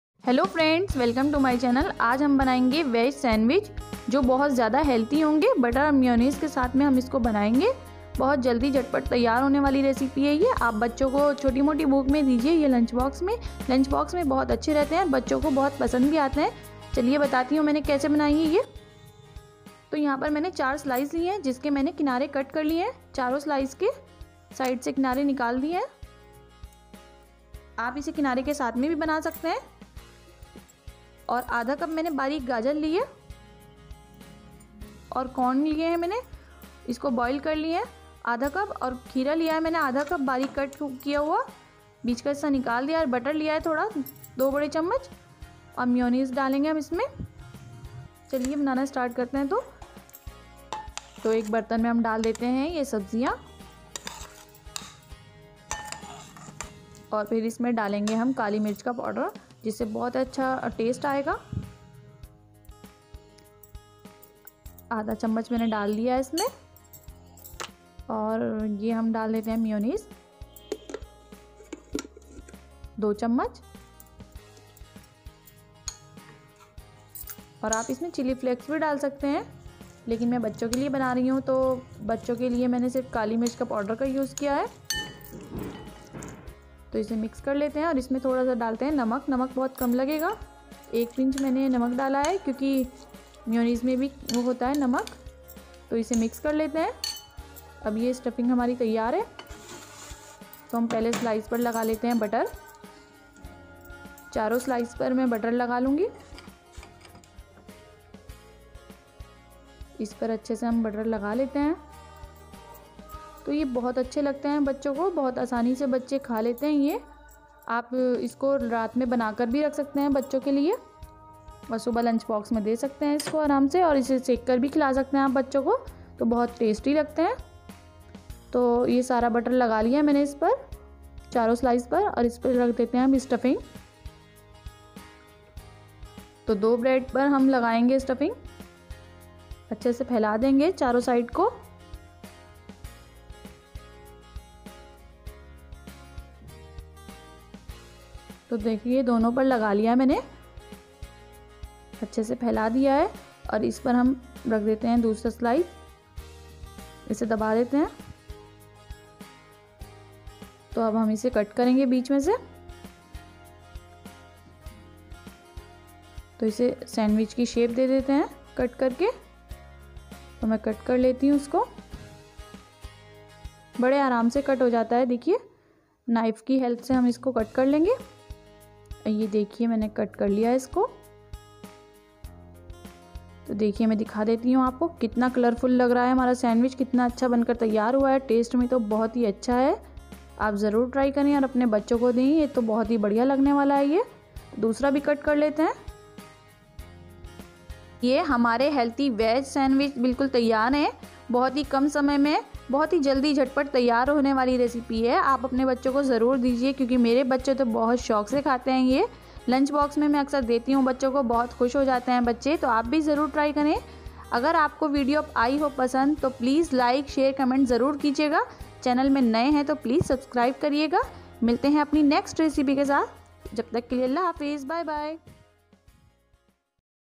बच्चो को पसंद आने वाले सेन्डविच बनाइयेलंच बॉक्स रेसिपीveg mayo sandwich रेसिपी
बच्चो को पसंद आने वाले सेन्डविच बनाइयेलंच बॉक्स रेसिपीveg mayo sandwich रेसिपी जो बच्चों को बोहोत ही पसंद आते हैं